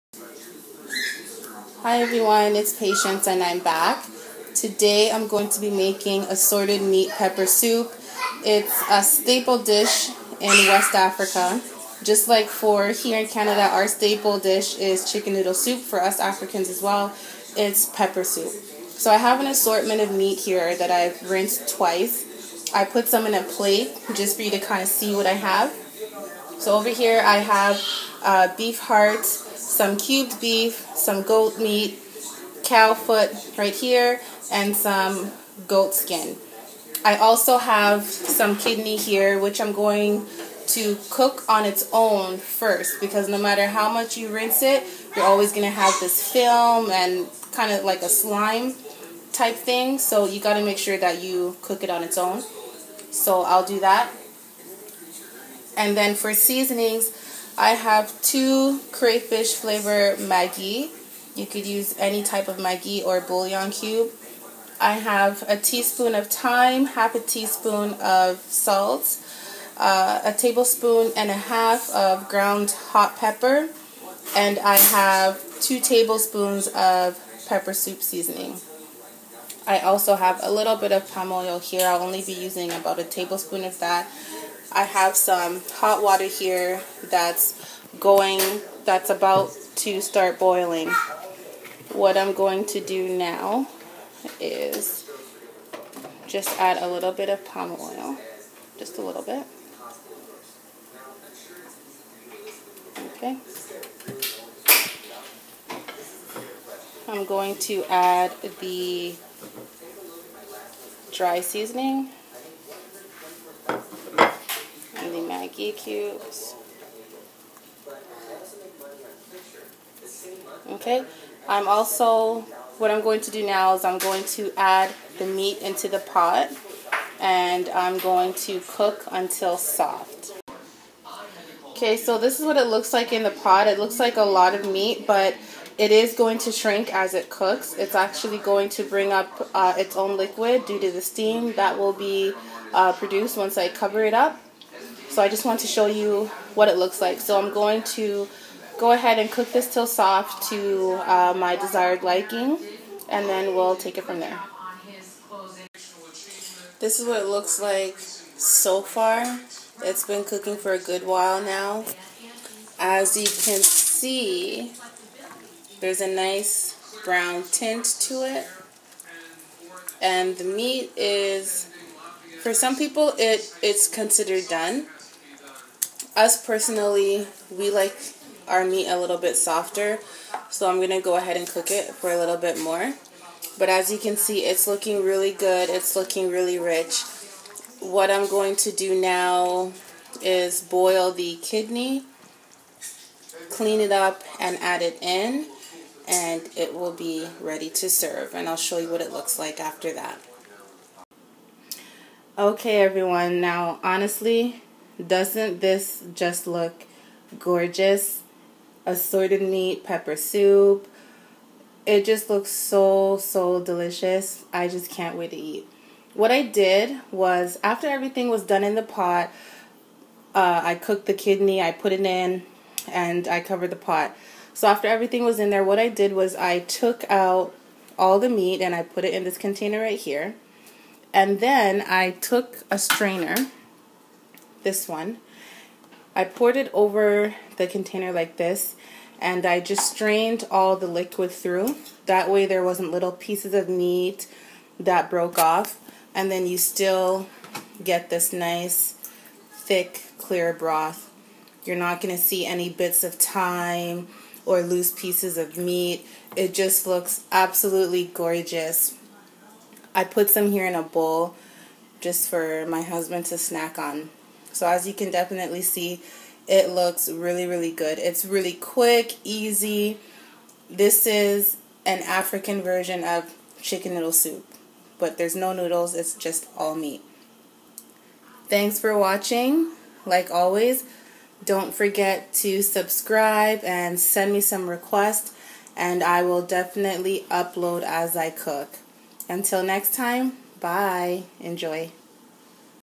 Nigerian Pepper Soup
Assorted meat pepper soup.  That explains it all lol. This deliciously spicy dish is served at every African party or get together.  That is a must!  It's also served to those who have just given birth, or to someone who isn't feeling well.  Nice tender meat and a broth to die for...who wouldn't love pepper soup? lol.  It can be eaten alone on a cold rainy day or on top of rice to complete a meal. Having it with pounded yam is also a popular way to serve this up.  This dish is so versatile and easily just as yummy! Join me as I show you my way of preparing this dish that will surely be a hit!